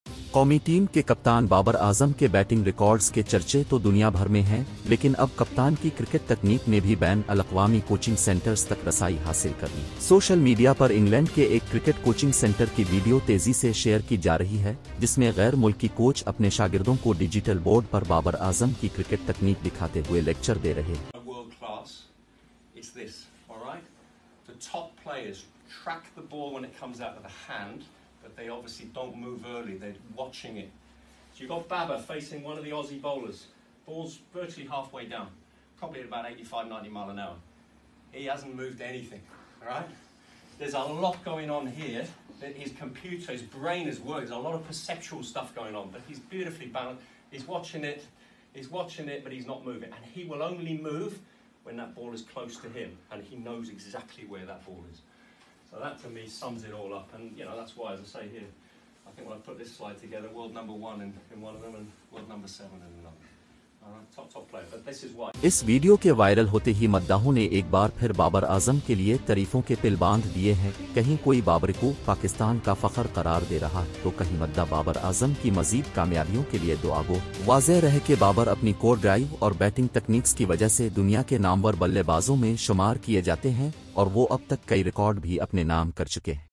Babar Azam Techniques Were Taught To Young European Cricketers
Young European cricketers began to be taught Babar Azam's techniques

The batting records of national team captain Babar Azam are talked about all over the world, but now the cricket technique of the captain has also gained access to international coaching centers.

A video of a cricket coaching center in England is being shared rapidly on social media, in which a foreign coach is lecturing his students on a digital board showing Babar Azam's cricket technique.

As soon as this video went viral, the fans have once again built bridges of praise for Babar Azam, some are calling Babar the pride of Pakistan and some are praying for Babar Azam's further success.

It should be noted that Babar is considered one of the most famous batsmen in the world due to his cover drive and batting techniques and he has set many records so far.